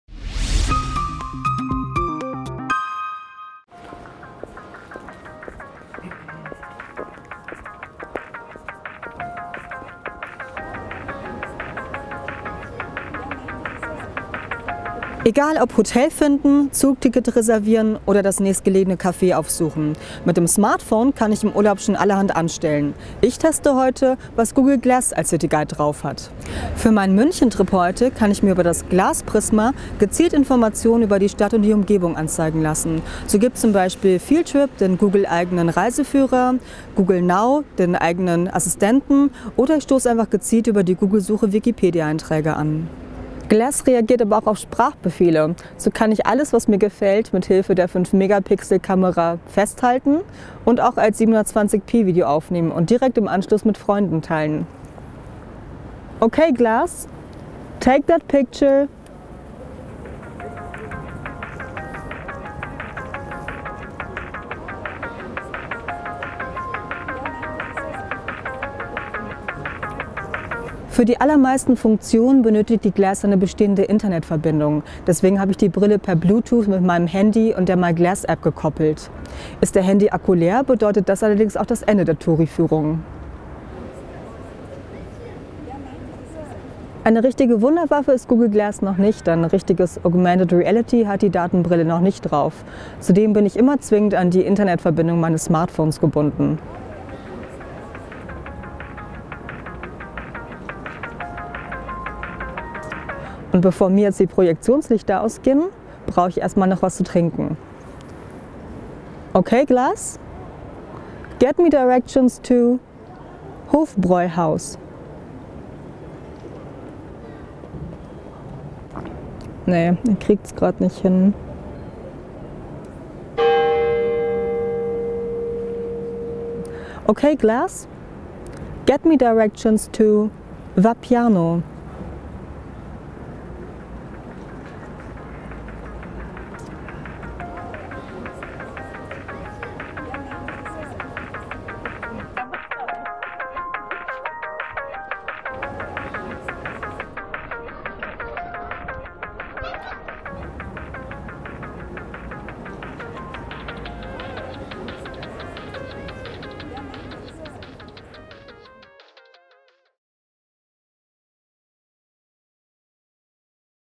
Google Glass - Praxis Test deutsch | CHIP
Städtetrip mal anders: Fotos von Sehenswürdigkeiten knipsen und die Wegbeschreibung zum nächsten Café abrufen, ohne dabei auch nur einmal das Smartphone oder Tablet zücken zu müssen. Google Glass macht's möglich. Was die Datenbrille tatsächlich kann und wie alltagstauglich sie ist, hat CHIP Redakteurin Beate Kipphardt ausprobiert. Unterwegs in München mit Google Glass - ein Praxistest.

Google Glass kaufen kann bislang aber nur ein ausgewählter Kundenkreis. Zugang zum rund 1.500 Dollar teuren Gadget gibt es bislang nur auf persönliche Einladung.

Preis Developer Edition: 1.500 US-Dollar (etwa 1.100 Euro)

Haben sie Fragen oder Probleme mit ihren technischen Geräten? Im CHIP Forum wird ihnen geholfen